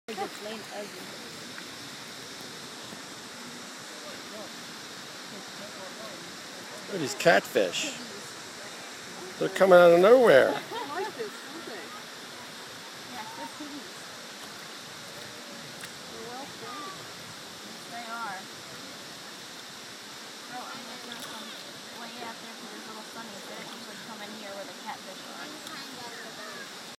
Cat fish
Feeding fish at longwood gardens